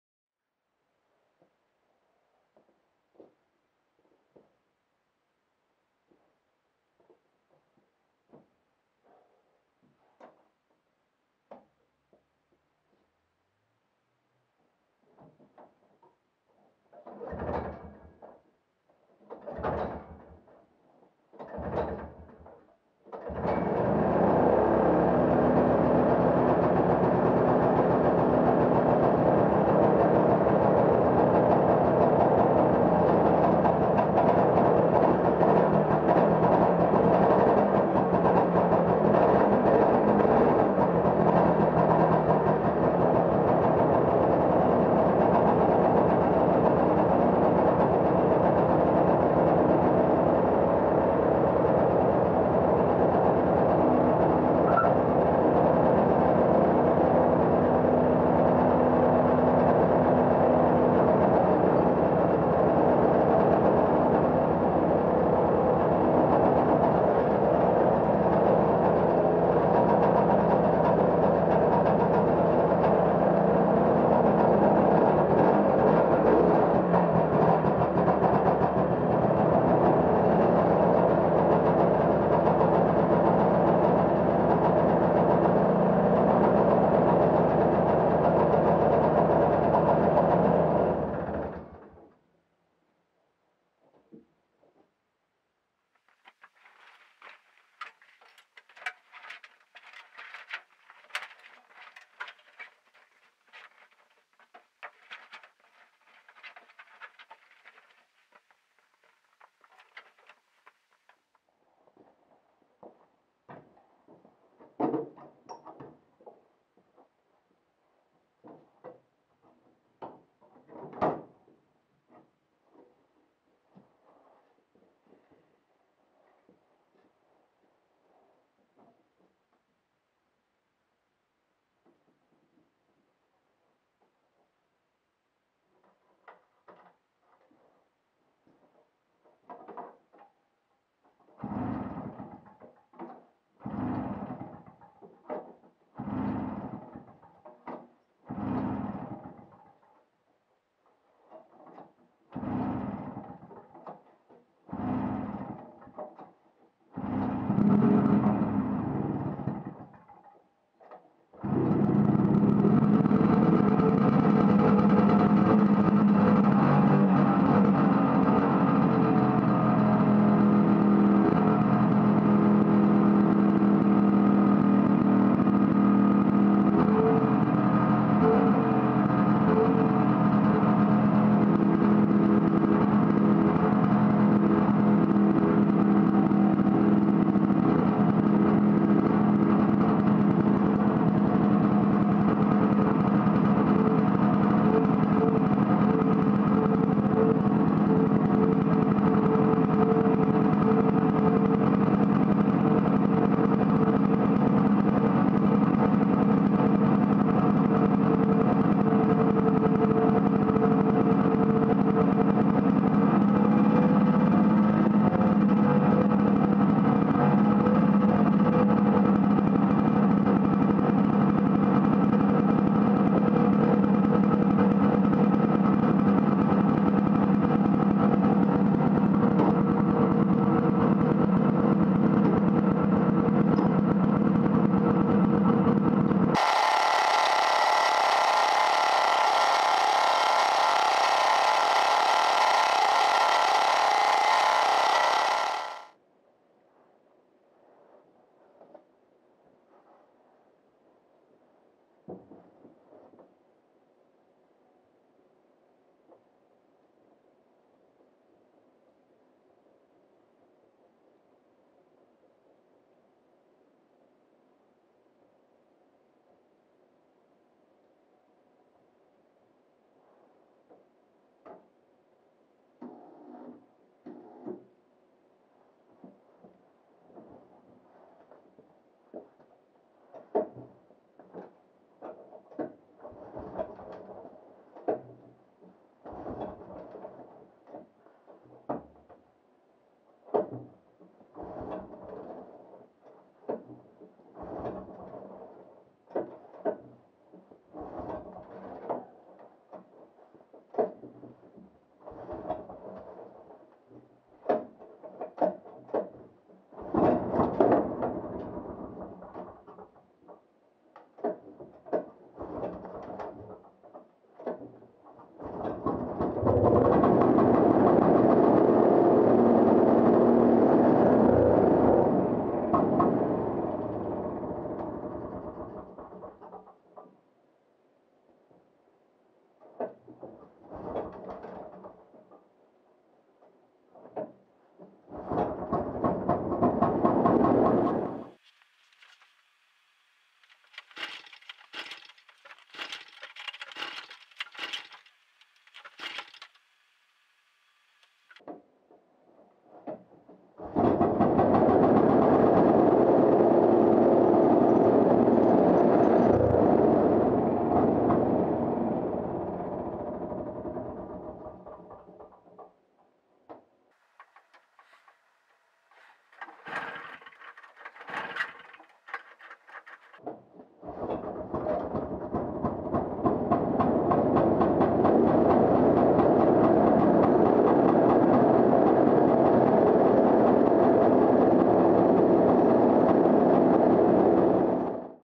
3 Vintage Snowmobile Cold Starts | Yamaha, John Deere, Polaris
1982 Yamaha Bravo
1973 John Deere 300
1968 Polaris Mustang